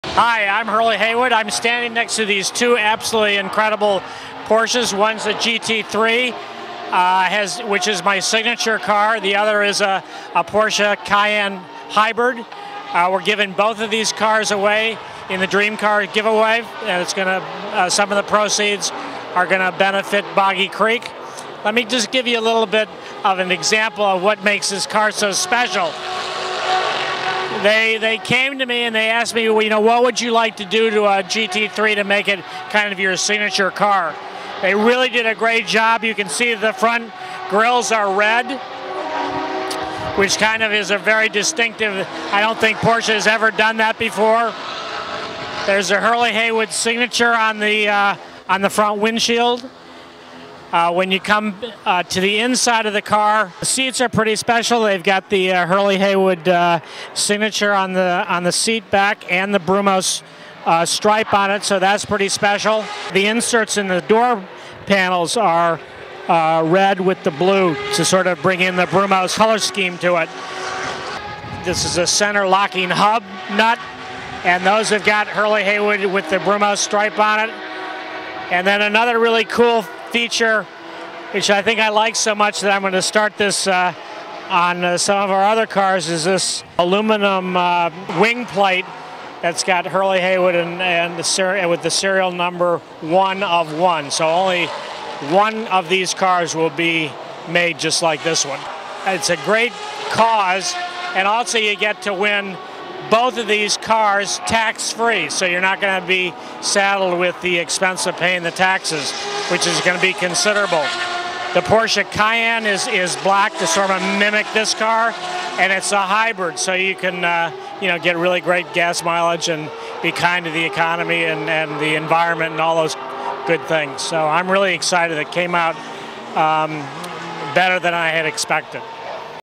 Continental Tire Dream Giveaway - Hurley Haywood
GRAND-AM and Continental Tire are giving away two 2011 Porsches to one lucky winner this year. Hurley Haywood shows off his custom touches to the GT3 and talks about the Cayenne S. Hybrid. For more information and to enter to win these two beautiful vehicles,